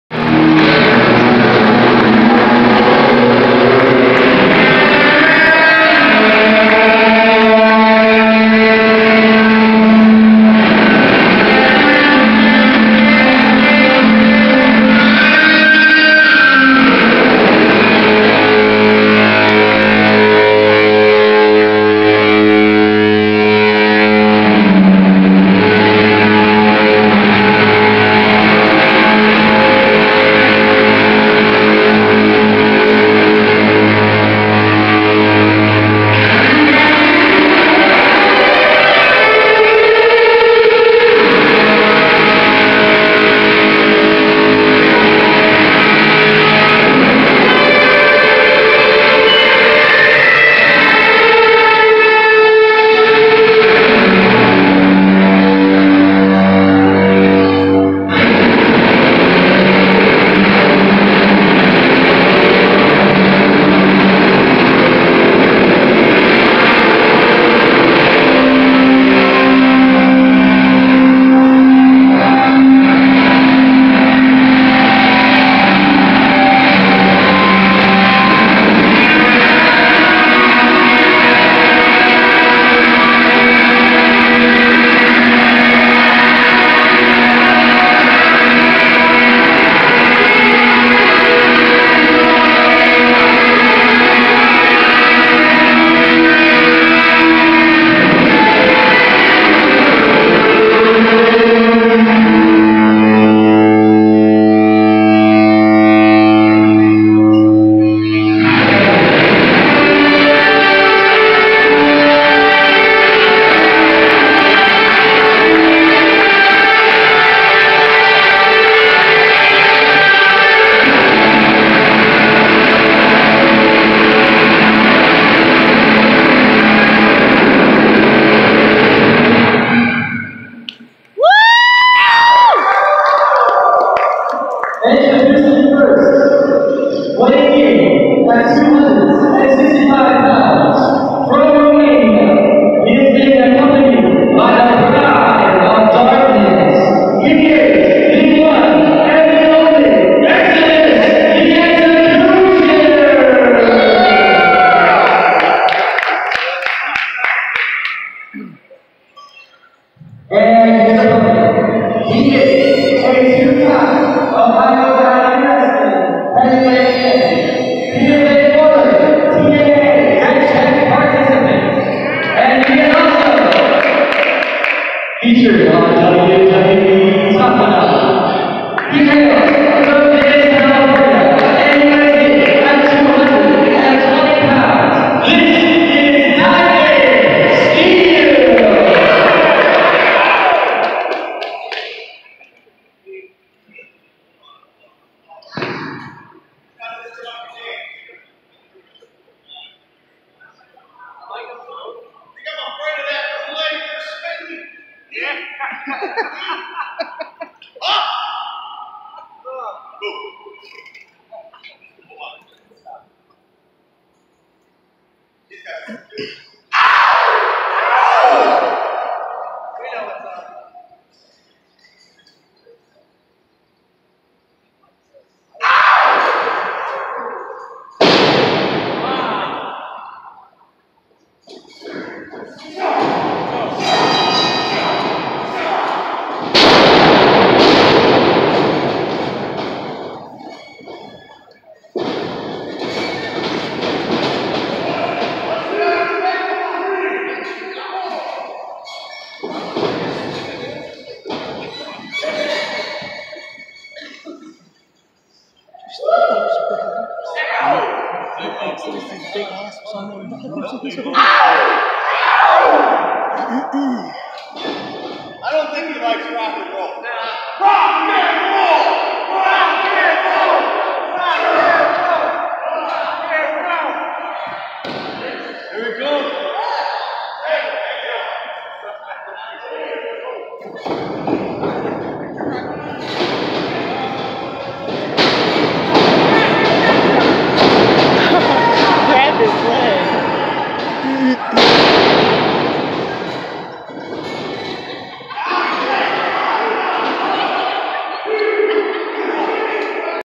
DIAMOND STEELE VS EXODUS "THE EXECUTIONER"
OSCW TAG TEAM WARFARE 9/30/2017.  MY PHONES SPACE RAN OUT :-(